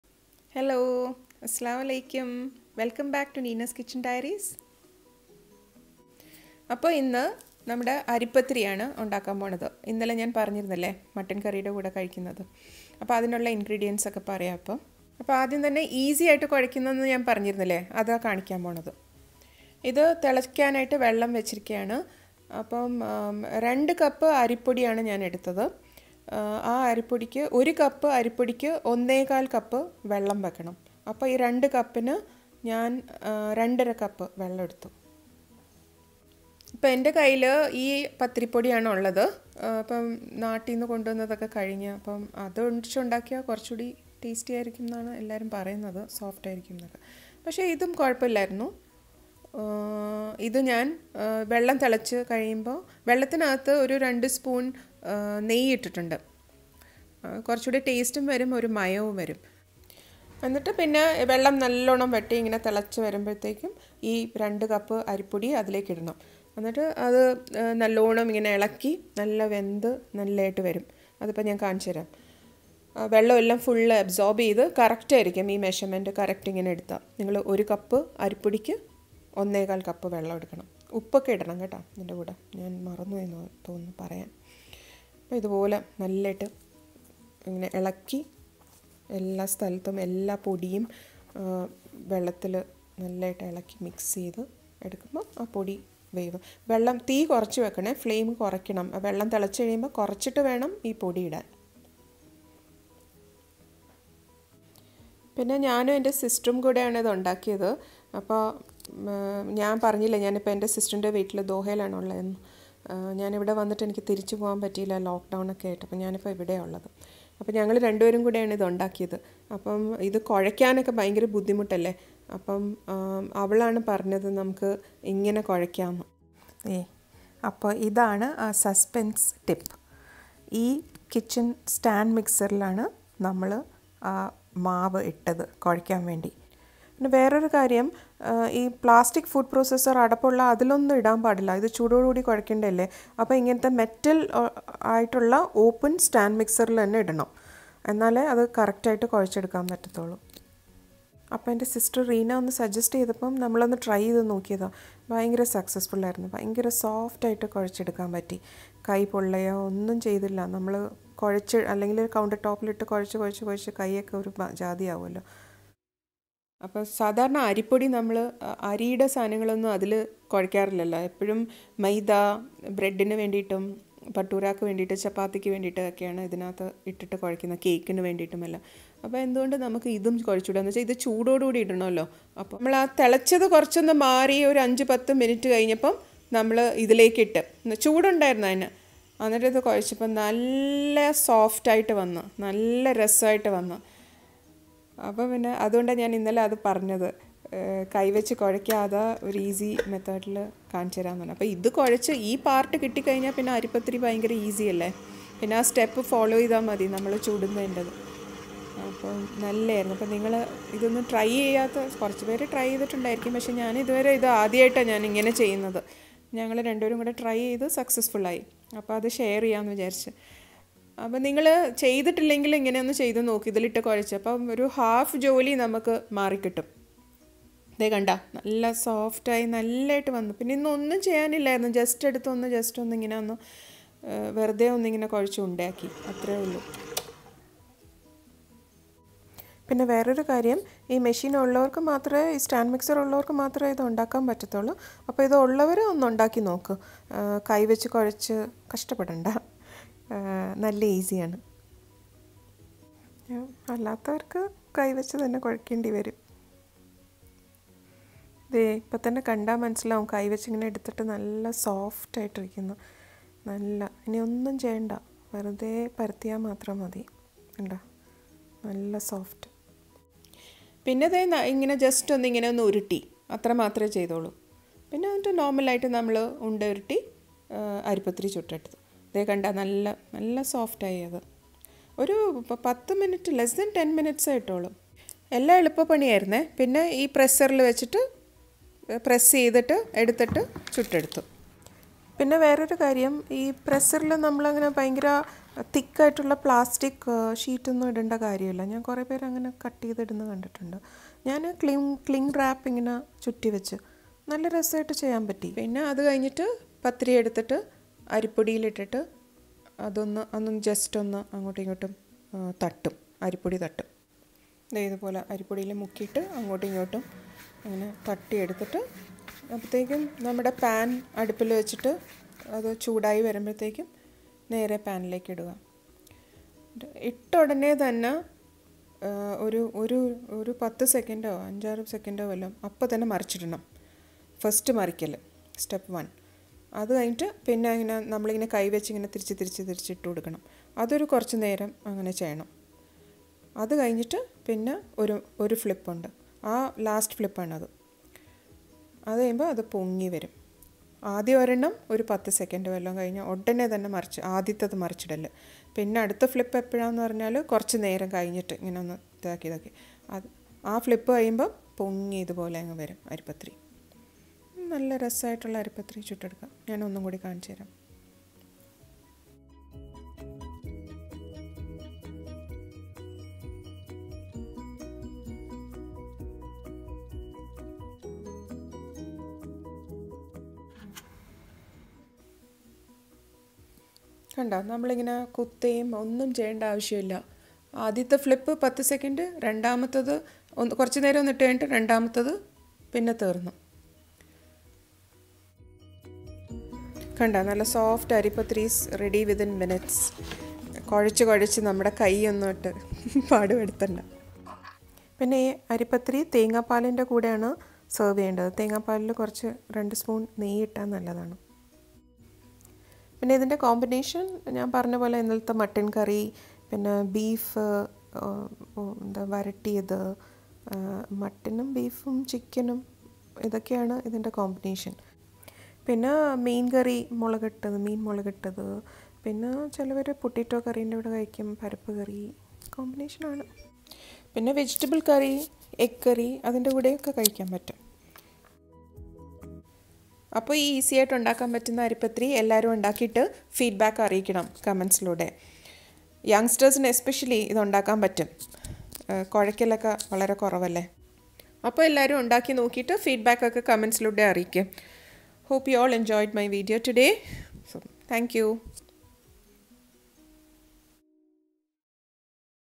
Kerala Ari Pathiri /Nice Pathiri made easy - Tips to save Kneading time Ramadan 2020 Series
How to make perfect Rice Pathiris (Kerala Special Rice Roti).  This is a regular roti made at most of our houses in Kerala.  I have made this video for those who are struggling (especially youngsters) to make perfect nice and soft pathiris at home.  This recipe yields around 30 pathiris.

Ingredients:
Roasted Fine Rice Flour.   -  2 cups
Ghee - 2 tsp (you may also add coconut oil)
Coconut milk for soaking the rice rotis
Preparation:  Step by step instructions as in my video

Neena